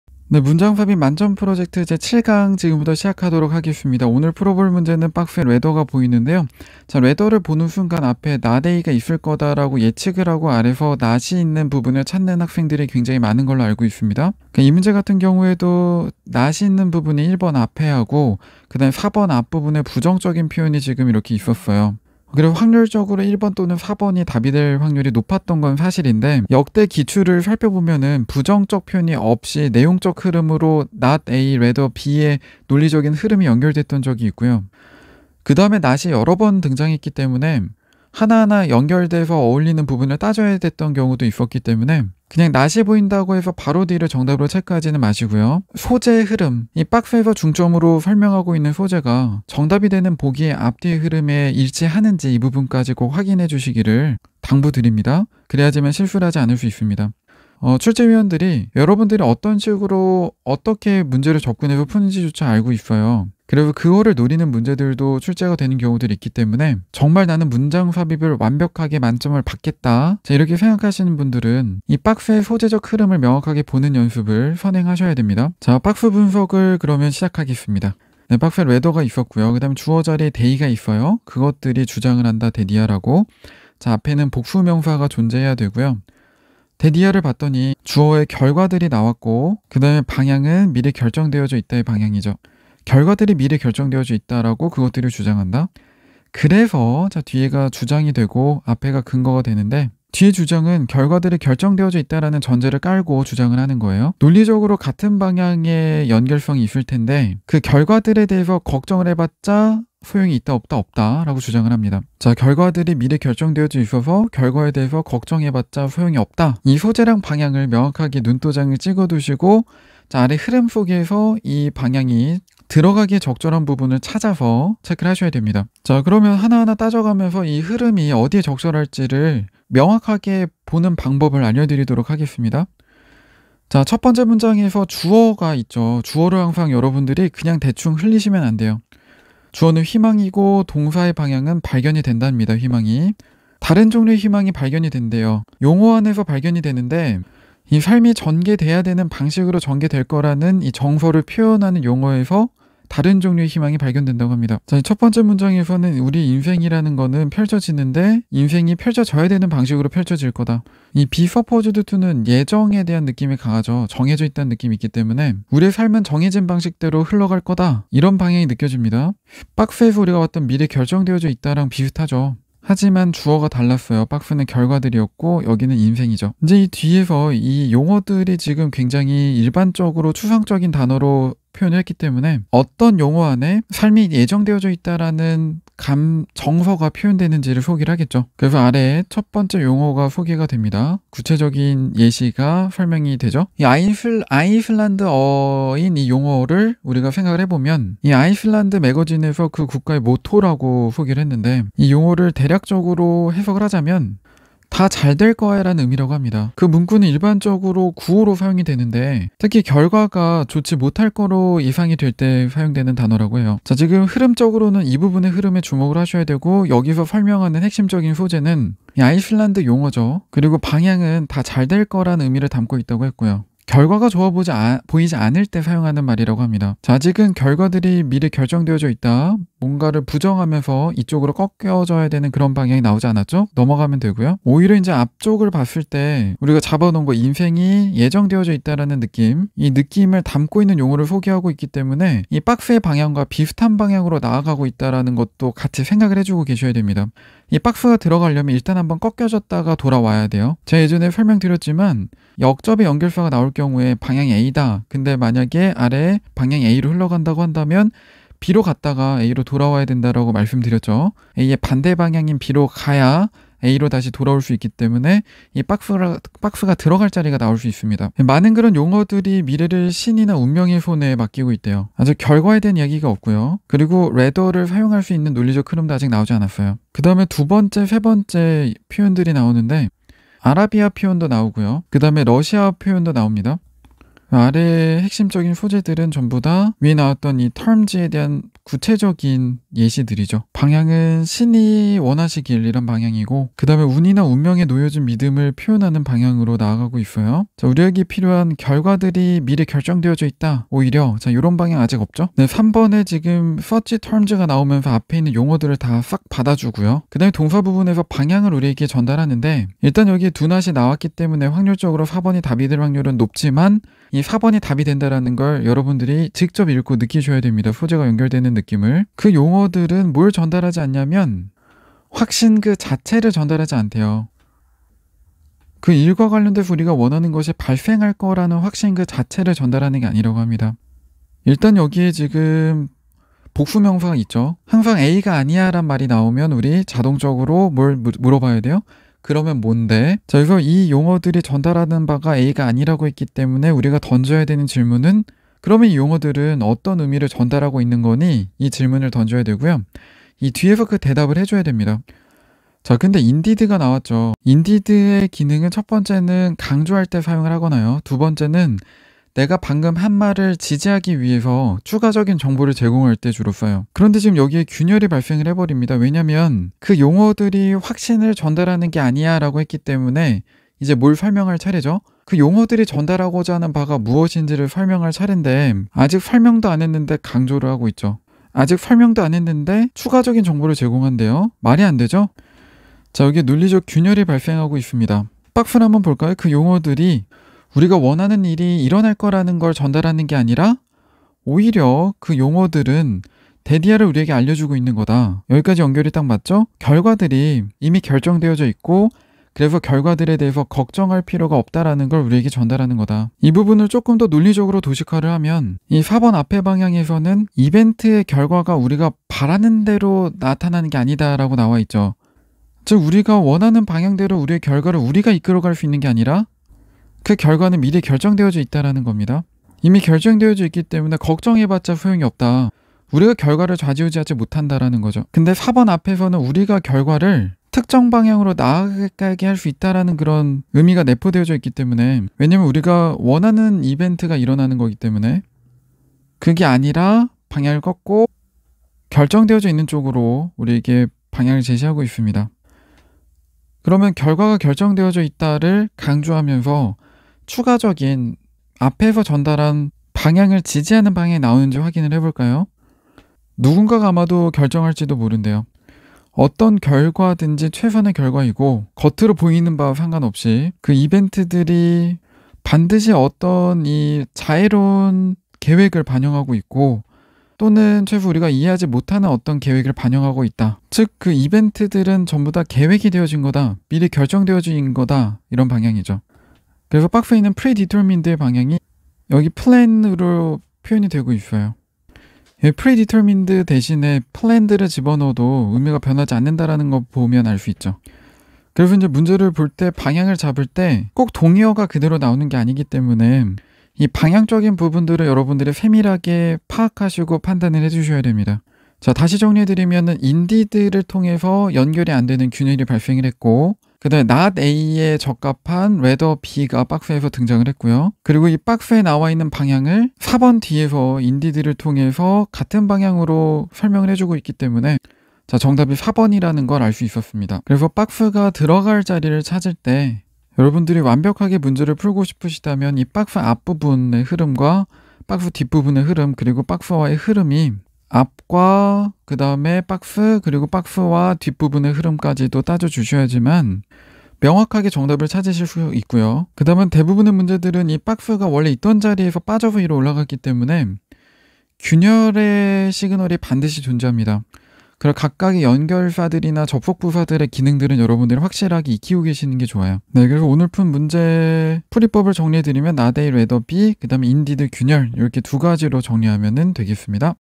수능 영어 문장삽입 수능 만점 프로젝트 7강/ 2022 EBS 수능완성 실전 모의고사 5회 38번/ 문장삽입 푸는 법
** 앞 강의들은 문장삽입 집중훈련 플레이 리스트에 있어요.

"여행의 진가는 수백 개의 땅을 같은 눈으로 바라볼 때가 아니라,
수백 개의 다른 눈으로 같은 땅을 바라볼 때 드러난다."

"The only true voyage would be not to travel through a hundred different lands with the same pair of eyes, but to see the same land through a hundred different pairs of eyes.
- Marcel Proust-